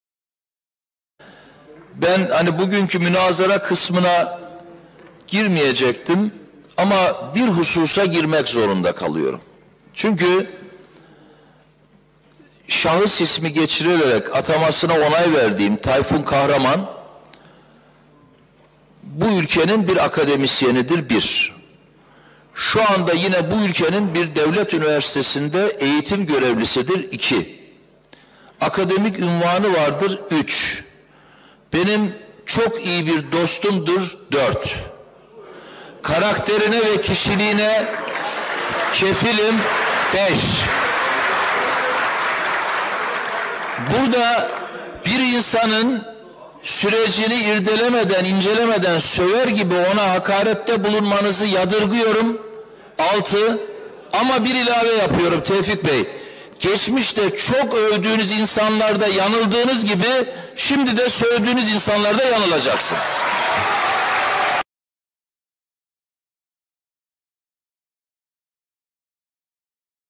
Ekrem İmamoğlu, Gezi Davası'nda 18 yıla mahkum edilen Tayfun Kahraman'a böyle sahip çıkmıştı!
İBB Başkanı Ekrem İmamoğlu, Gezi Davası'nda 18 yıla mahkum edilen Tayfun Kahraman'a böyle sahip çıkmıştı!